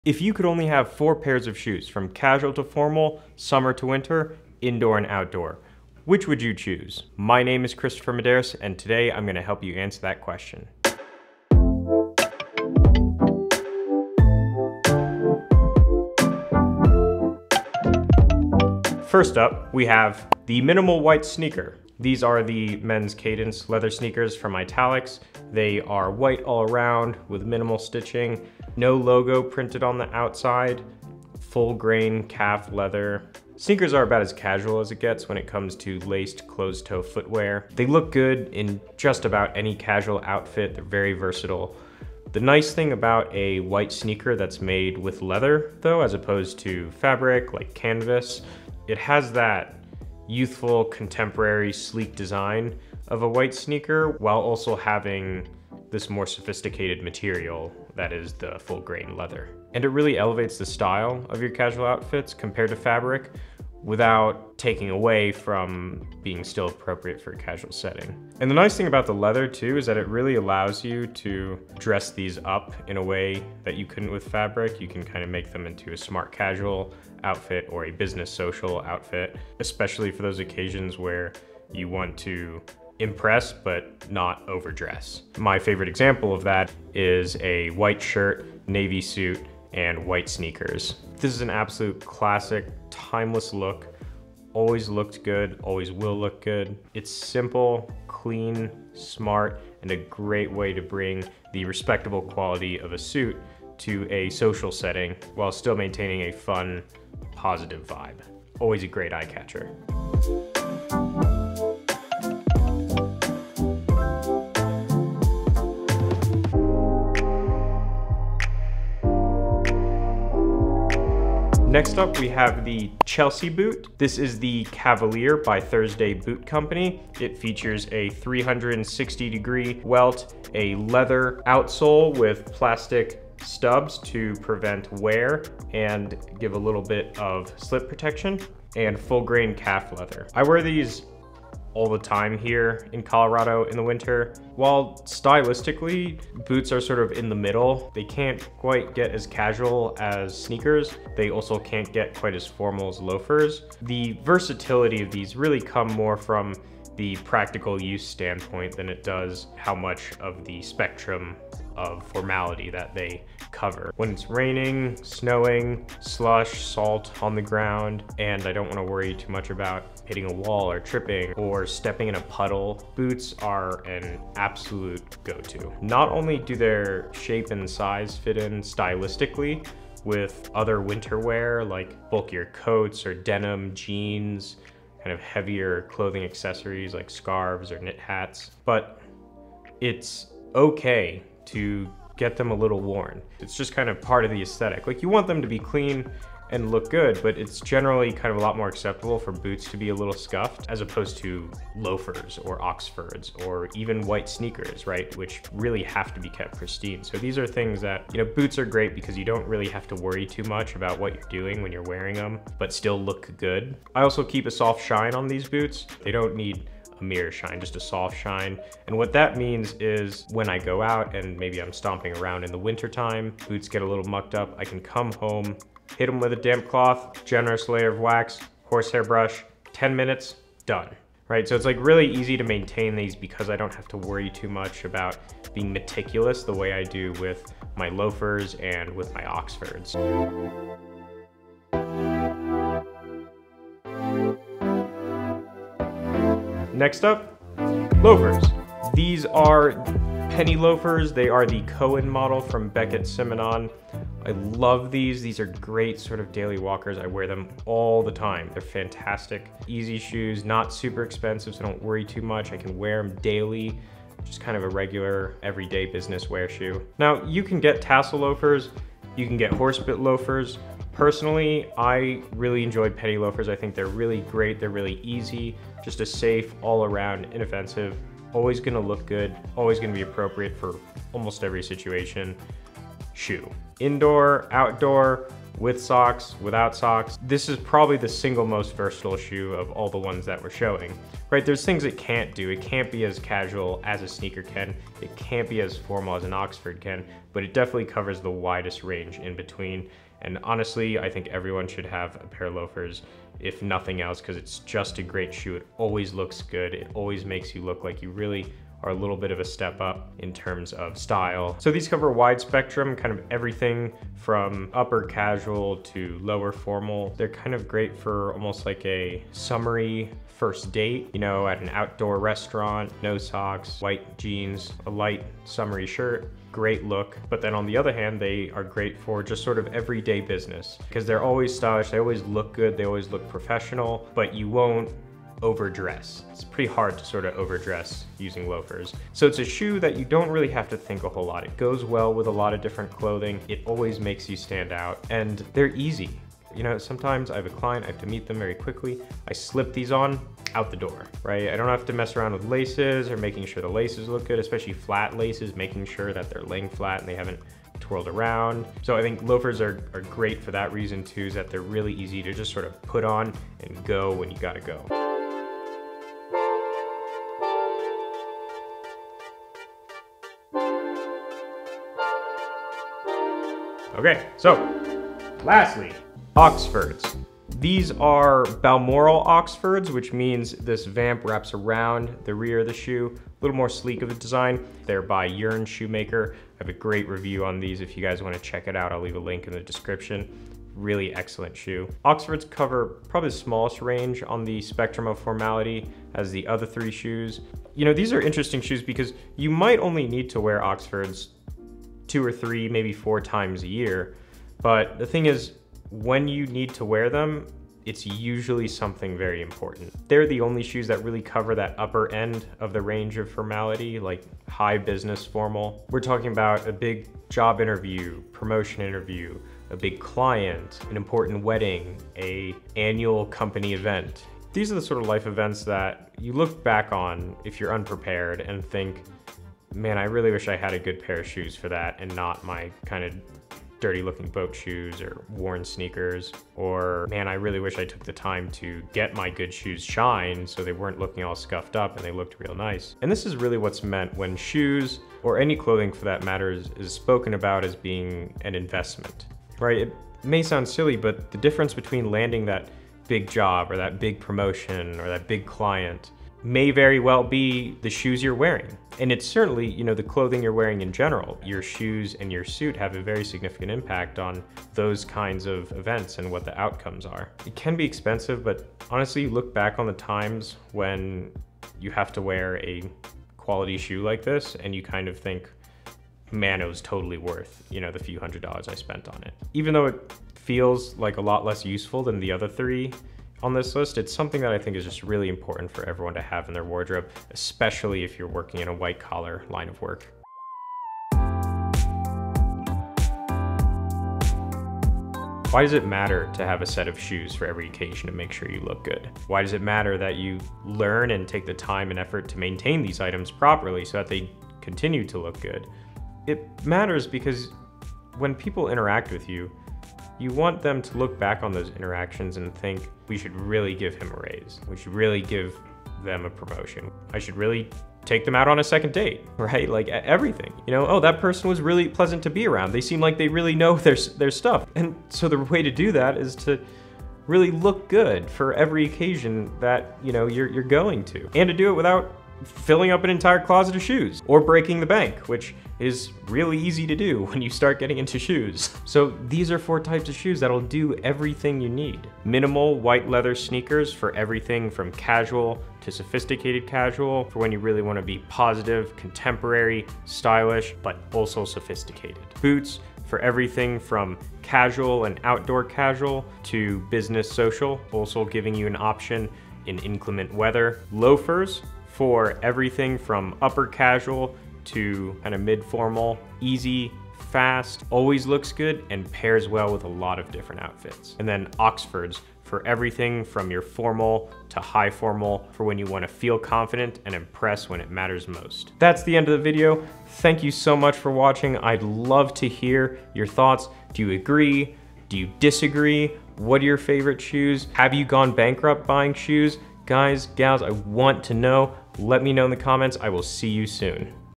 Why These Are The 4 Shoes For Every Occasion.
If you could only have four, which would you choose?

In this video we take a look at four pairs of shoes and why & how they constitute the essentials of every man's wardrobe. You need to cover everything from Casual to High Formal, but without filling an entire closet. We talk about material, outfits, use, and versatility - everything you need to know about the basics of covering the entire spectrum of formality with your footwear.

\\\ 🌍 Products Mentioned \\\
White Leather Sneakers (Italic no longer offers these, I suggest Beckett Simonon)
Chelsea Boots (Hickory)

\\\ ⌚️ Timestamps \\\
00:00 - Introduction
00:18 - White Leather Sneakers
02:00 - Chelsea Boots
04:00 - Penny Loafers
06:11 - Oxfords
08:13 - Recap

\\\ 🖼️ The Painting \\\